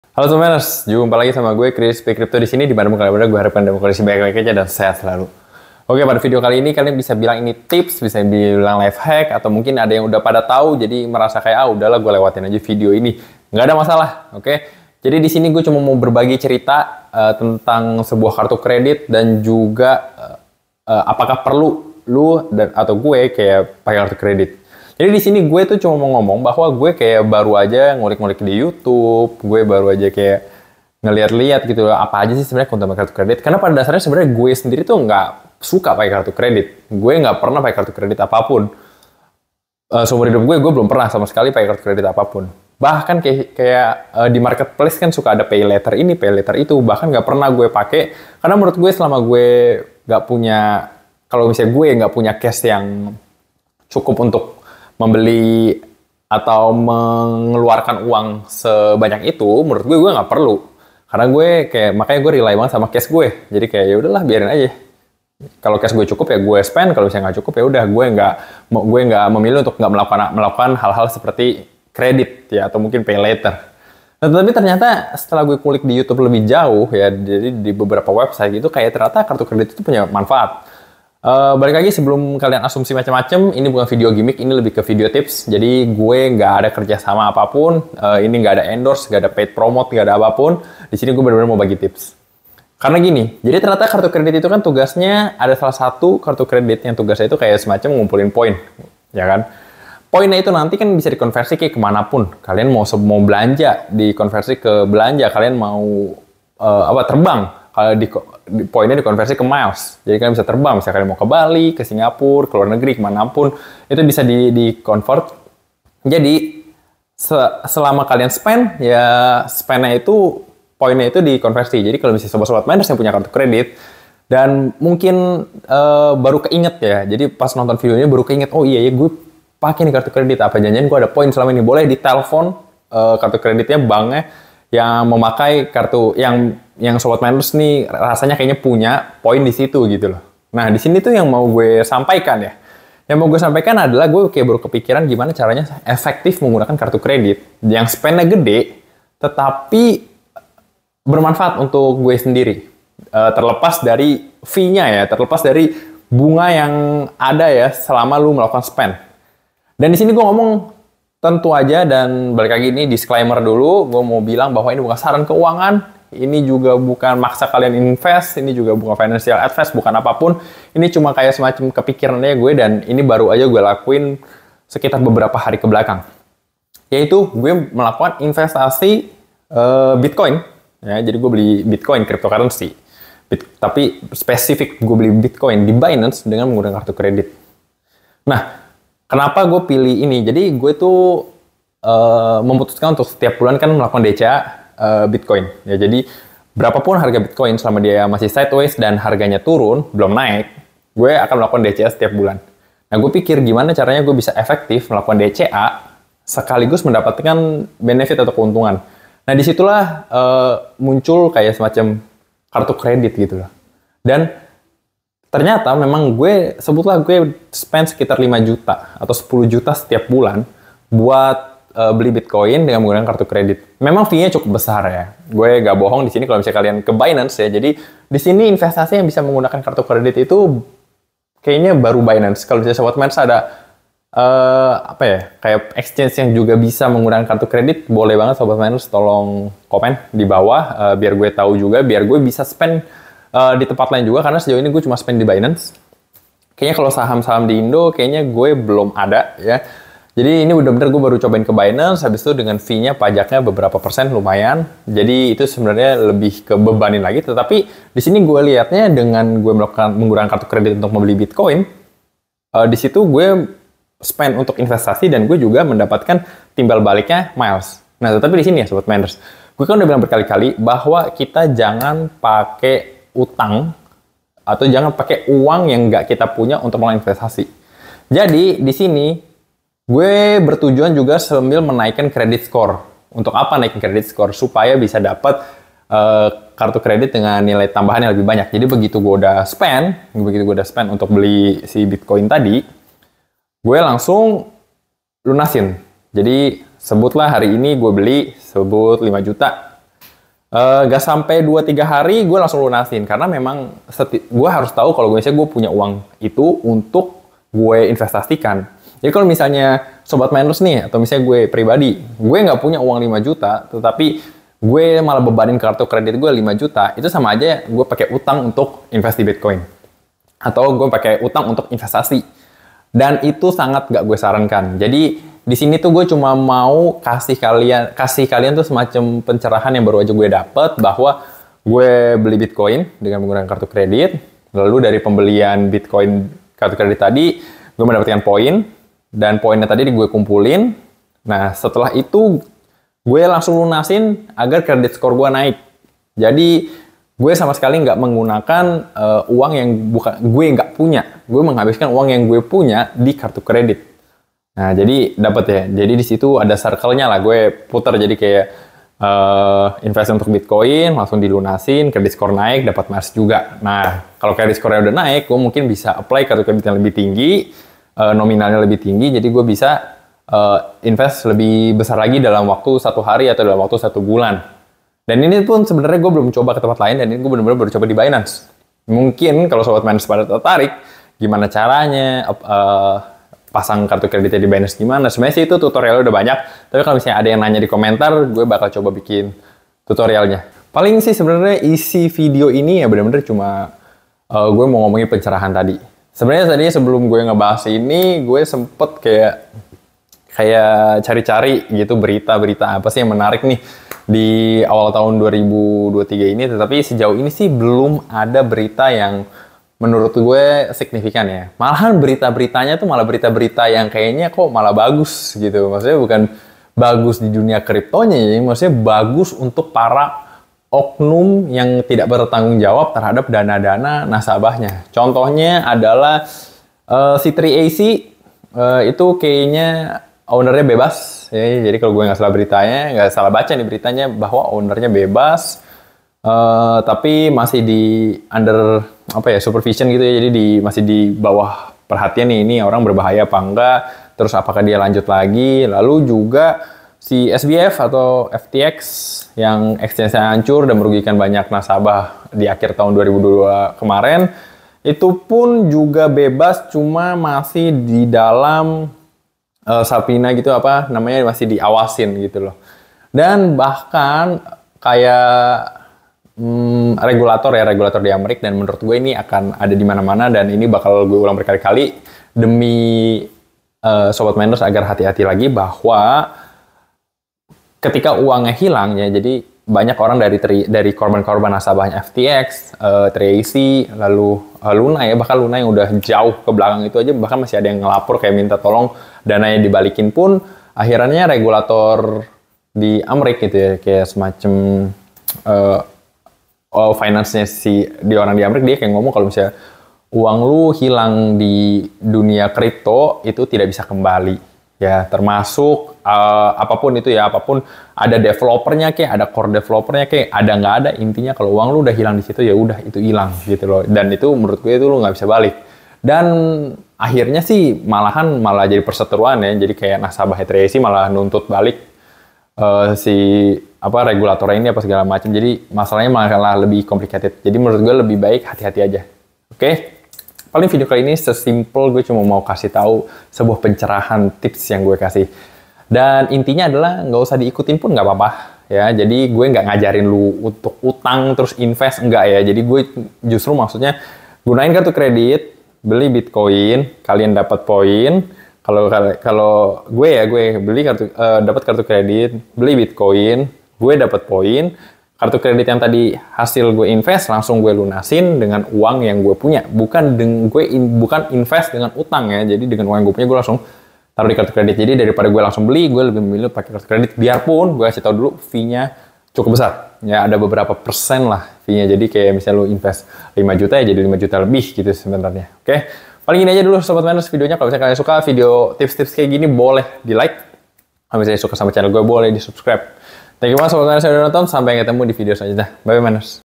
INVESTASI CRYPTO PAKE KARTU KREDIT AJA ! GRATIS TERBANG KE LUAR NEGERI !
Ini adalah hasil ngulik gue selama beberapa bulan kebelakng, dan gue merasa ini menarik untuk di bagikan ke sobat miners, enjoy !

Gue juga bikin artikel - artikel tentang pengalaman mining gue bisa kalian buka disini :

REFFERAL CODE BYBIT :
47569